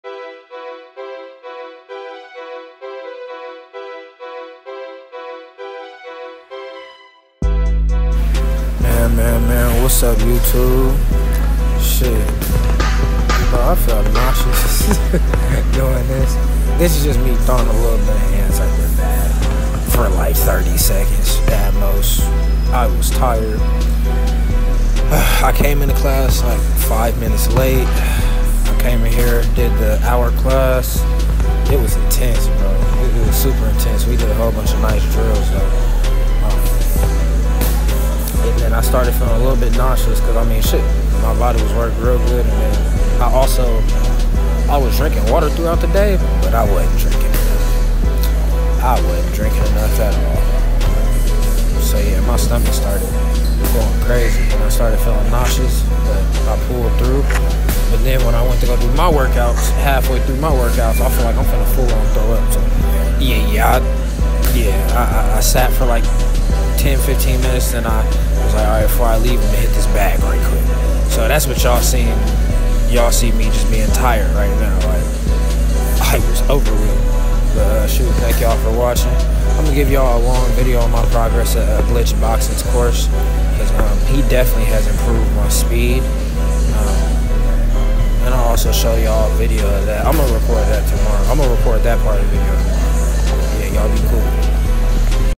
Those last seconds before you leave the boxing gym
By this time I had did an hour class and half of my workouts and I started feeling nausea so I just decided to hit the bag a lil and gtfo.